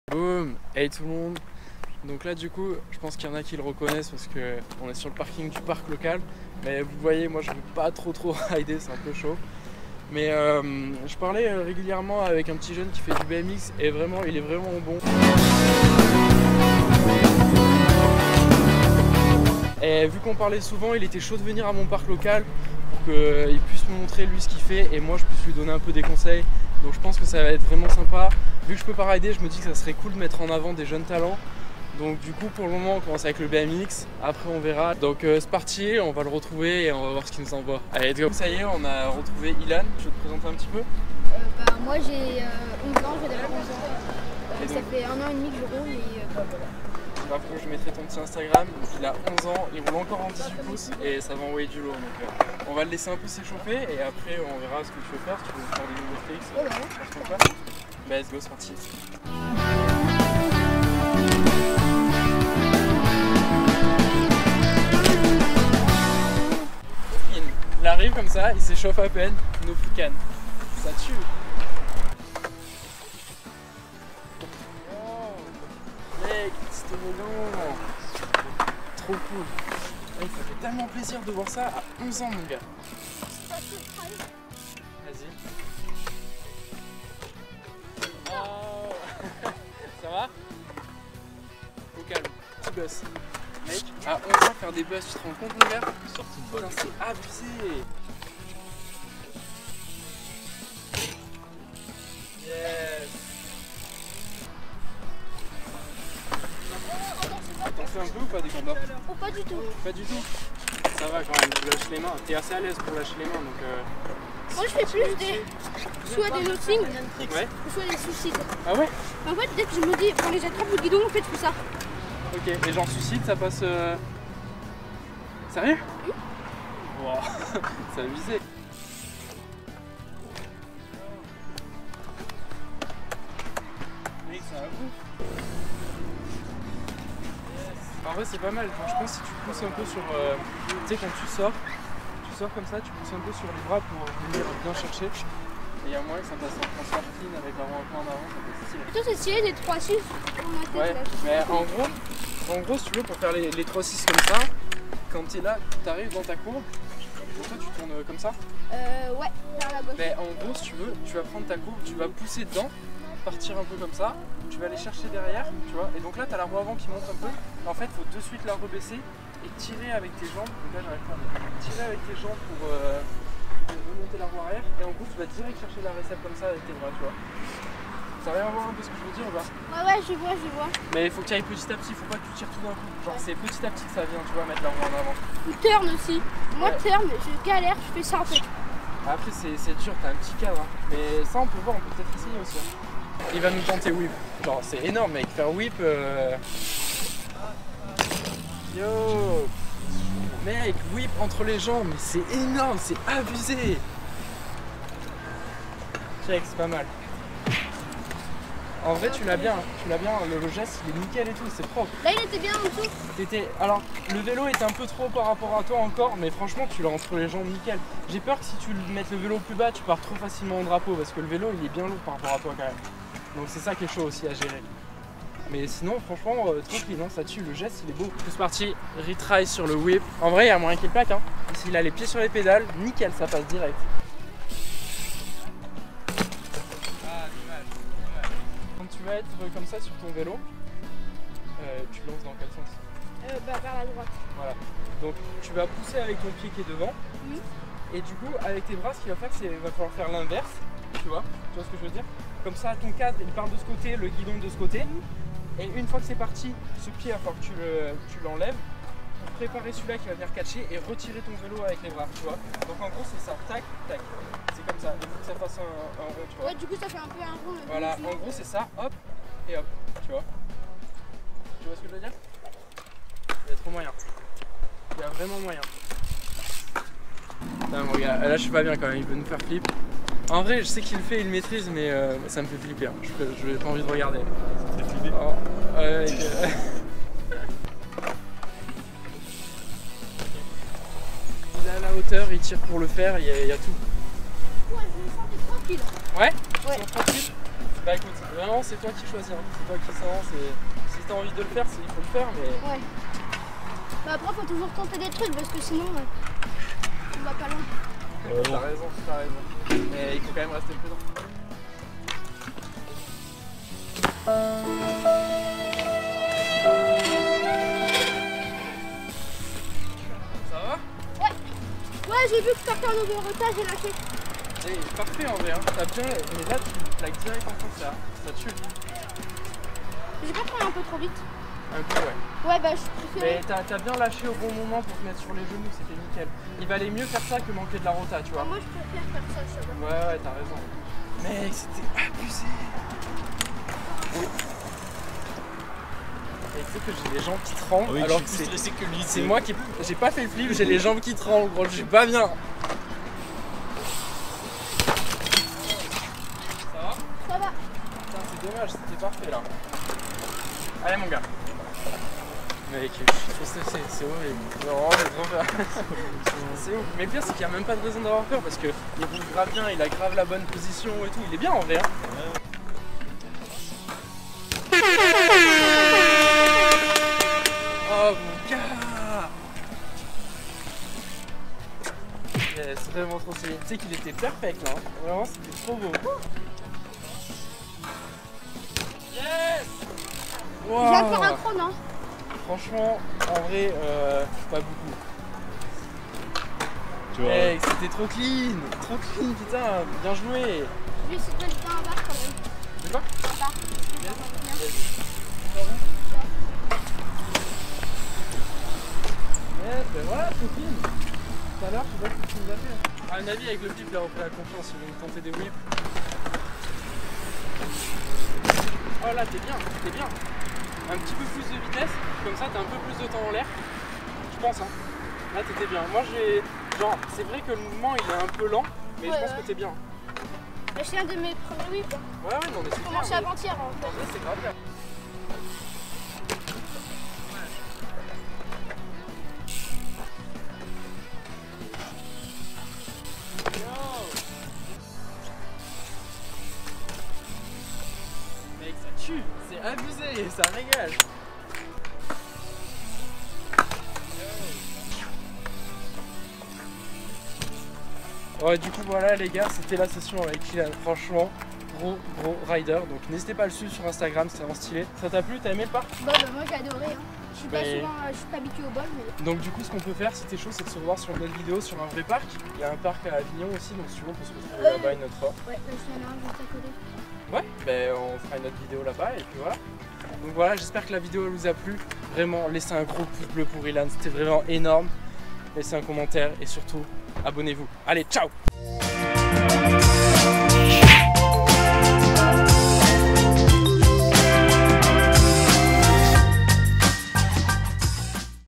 IL FAIT BACKFLIP EN BMX À 11ANS !!
Grosse session pour ilan en bmx au bowl de ile piot, skatepark à avignon !
Du gros niveau de ride !

Continuez, persévérez et n'abandonnez jamais !